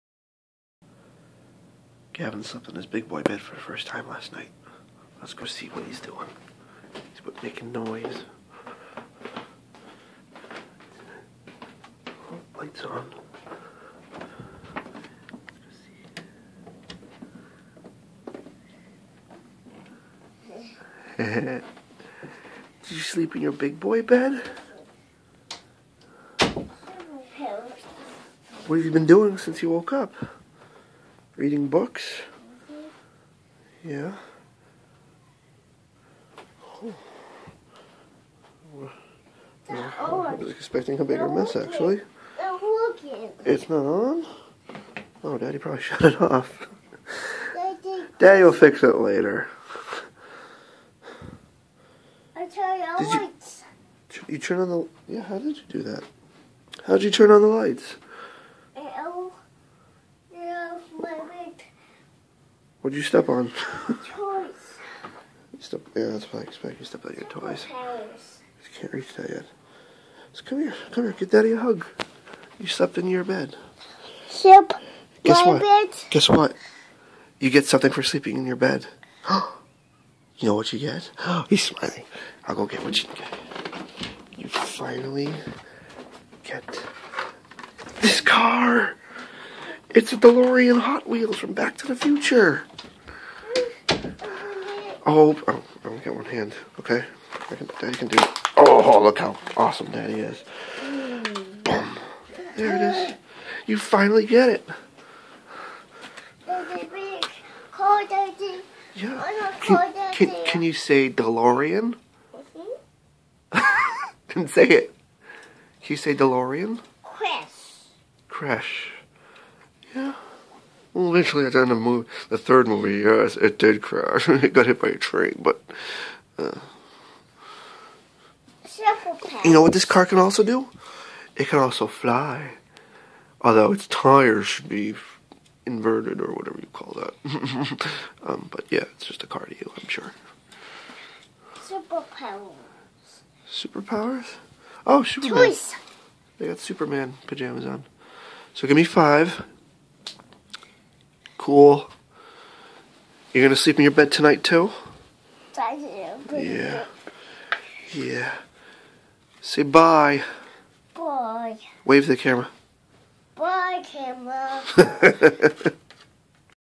598 - Gav Sleeps in his Big Boy Bed (1st Time)
We've been trying to get Gavin to sleep in his big boy bed, but he didn't want to. We tried every night, until one day I just took the mattress out of his crib, and he had no choice but to sleep in his new bed. I had to lay down with him, but he eventually went to sleep, and we had successfully gotten him into his big boy bed. :)